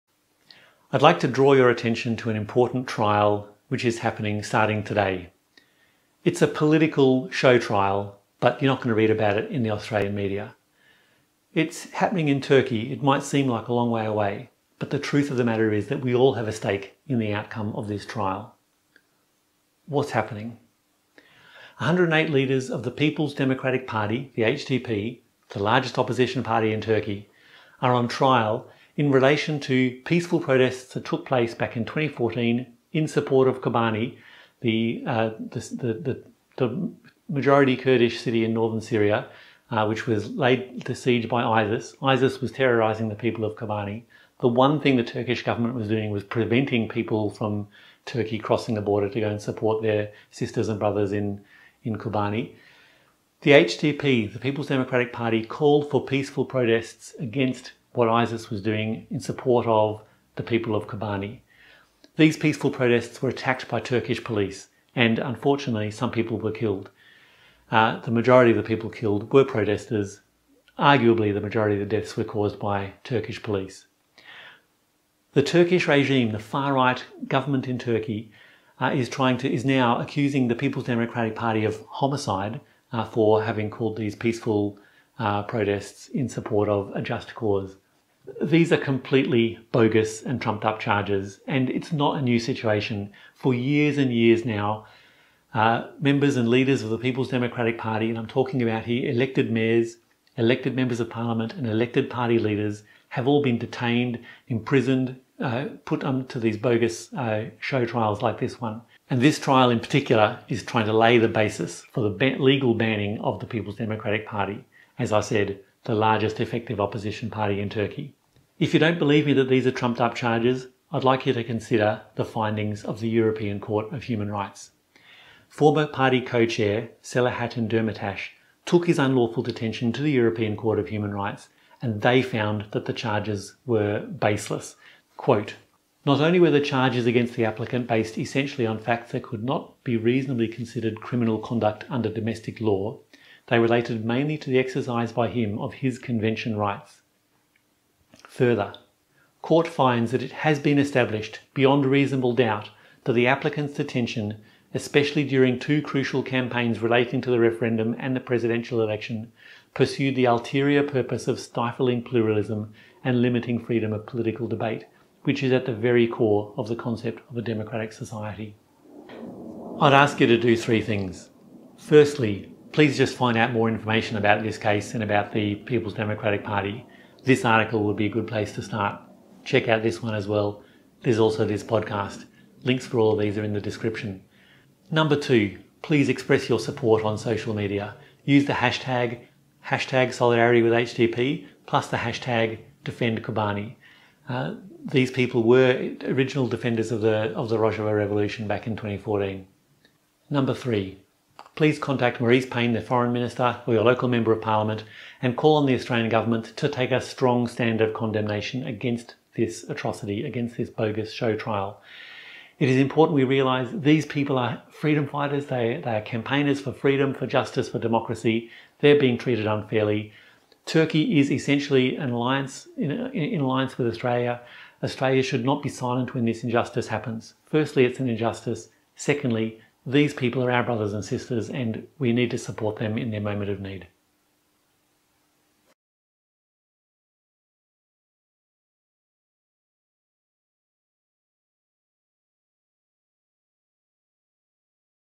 Condemn the bogus show trial of Turkish HDP leaders
Alex Bainbridge from the Socialist Alliance Australia expresses solidarity with the Peoples' Democratic Party (HDP) in Turkey who are facing a bogus show trial that could result in the party's banning.

Please help by:
1. Find out more information (as above)
3. Contact foreign minister Marise Payne and call on the Australian government to publicly condemn this show trial: